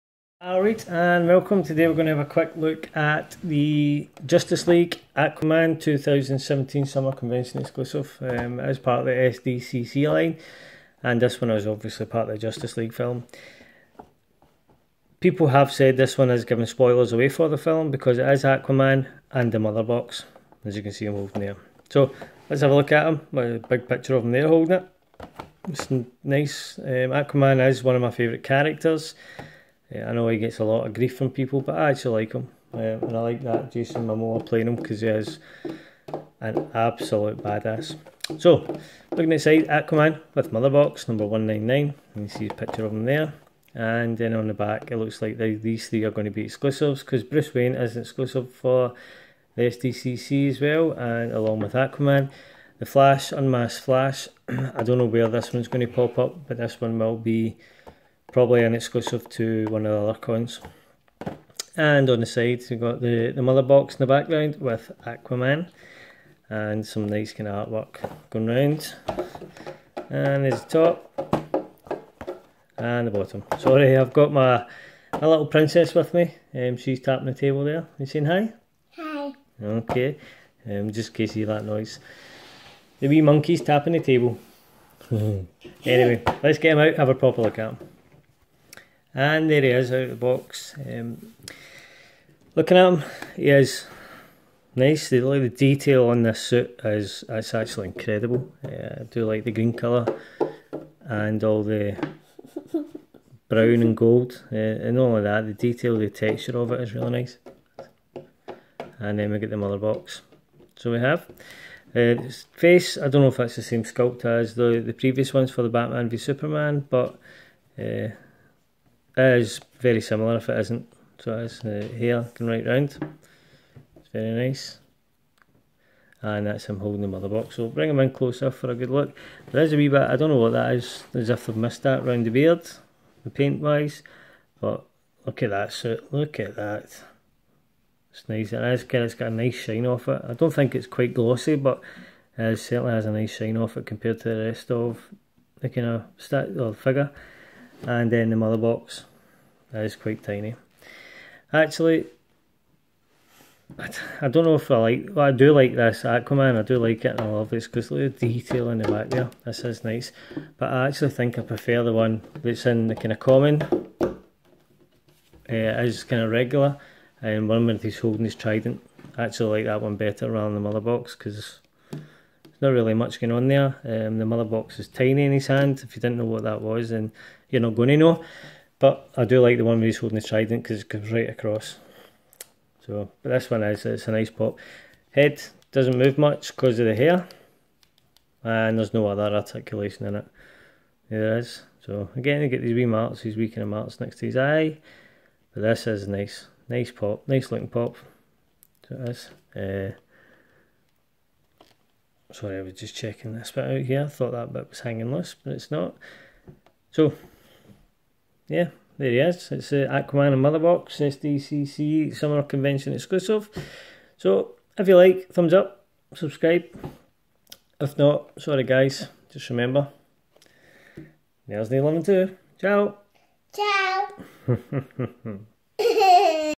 SDCC 2017 Exclusive Aquaman with Motherbox Funko Pop Unboxing
A quick unboxing and review of the SDCC 2017 Aquaman funko pop